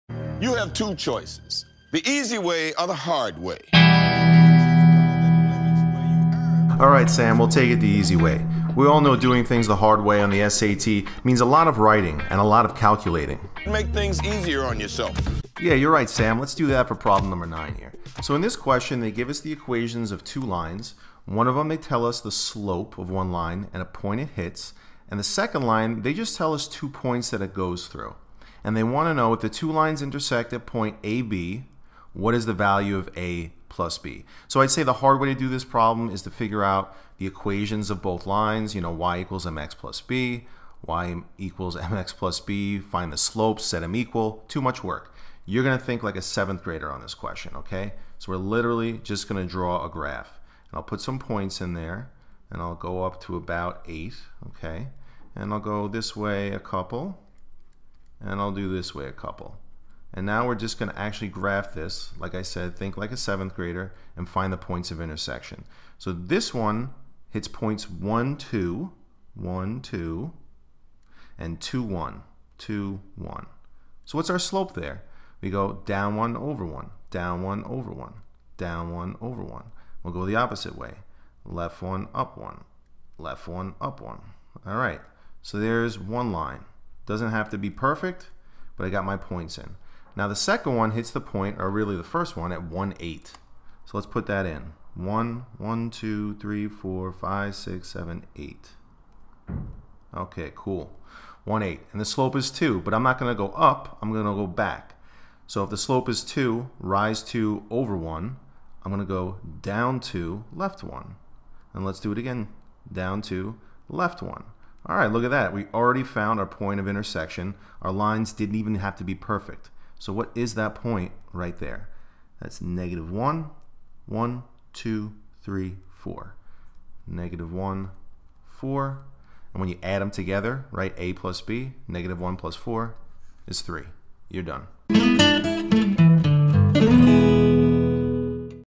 New SAT Math -Easy Way, Hard Way Series #3
New Sat Tips Tricks and Strategies for doing hard problems the easy way! Not the Hard way. Plenty of difficult problems can be solved using these hints and tips. Check out this great series!